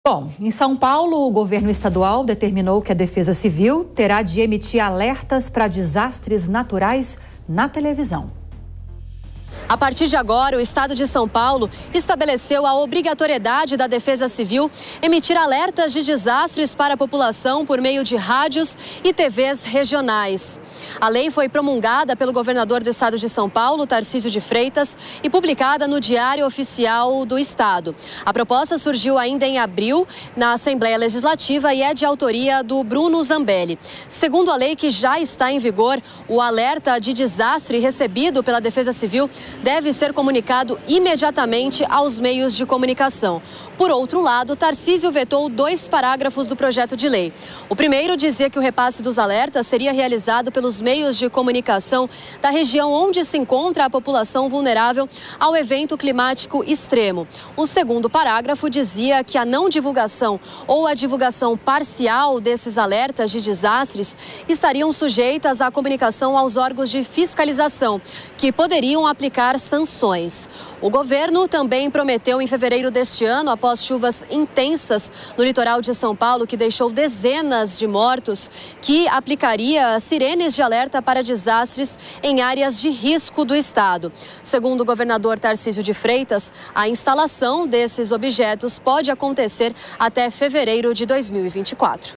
Defesa Civil terá que transmitir alertas de desastres naturais em TV e rádio em São Paulo
O governo de São Paulo determinou que a Defesa Civil do estado deverá emitir alertas de prevenção para desastres naturais em TV e rádio. A lei prevê repasses imediatos a meios de tele e radiodifusão.